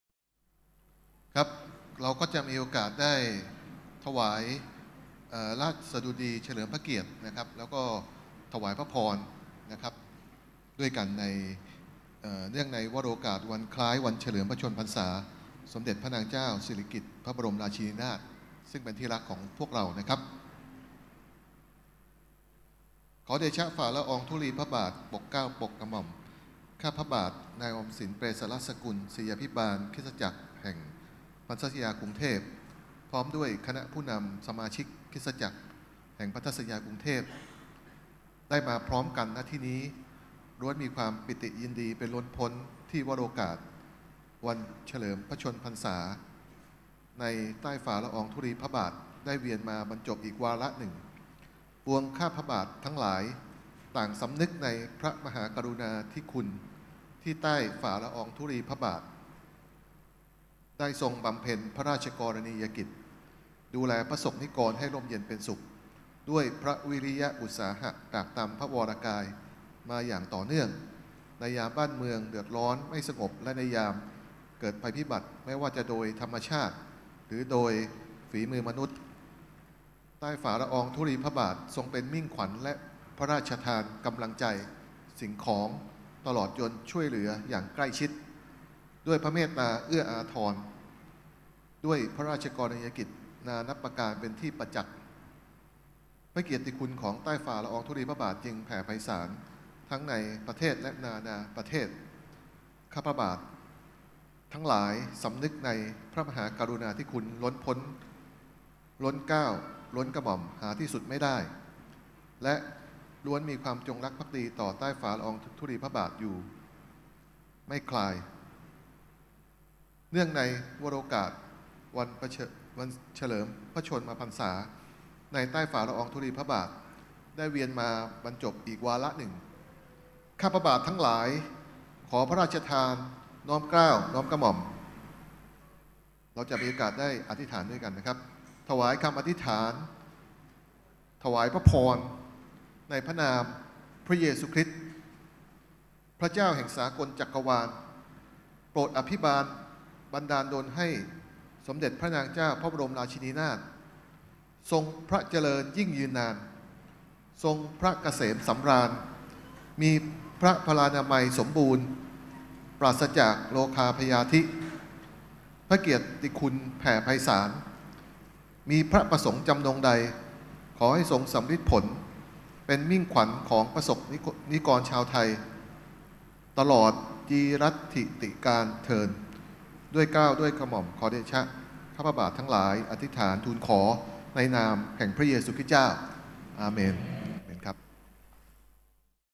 งานวันแม่ 2015 อธิษฐานถวายพระพร
งานวันแม่ 2015 "Everyday is MOM, ทุกวันฉันมีเธอ" ณ คริสตจักรแห่งพันธสัญญากรุงเทพ